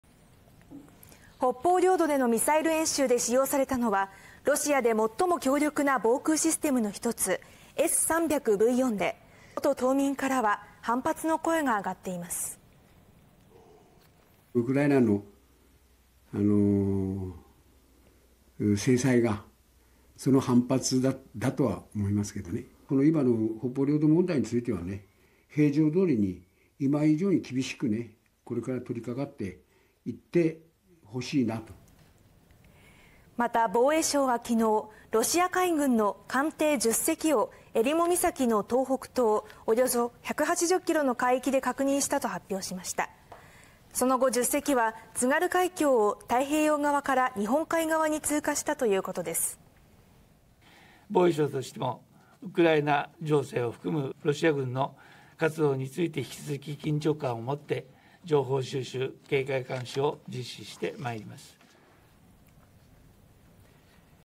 北方領土で"ミサイル発射"演習…元島民から反発の声 ロシア海軍「津軽海峡」通過も確認 制裁へけん制か (22/03/11 18:45)
ロシアによるウクライナ侵攻が続くさなかの3月10日、ロシア国防省は北方領土でミサイルの発射演習を実施したこと明らかにしました。

　北方領土の元島民からは反発の声が上がっています。

　北方領土でのミサイル演習で使用されたのは、ロシアで最も強力な防空システムのひとつ「S300V4」で、ウクライナ侵攻をめぐり経済制裁に加わった日本をけん制する狙いもあるとみられます。

　この事態に、北方領土の元島民からは反発の声があがっています。

　歯舞群島 勇留島出身 角鹿 泰司さん：「ウクライナの制裁の反発だと思う。いまの北方領土問題については厳しく取り掛かってほしい」

　また、防衛省は10日にロシア海軍の艦艇10隻を襟裳岬の東北東、約180キロの海域で確認したと発表しました。

　その後、10隻は津軽海峡を太平洋側から日本海側に通過したということです。

　岸 信夫 防衛相：「ウクライナ情勢を含むロシア軍の活動について、引き続き、緊張感を持って情報収集、警戒監視を実施していく」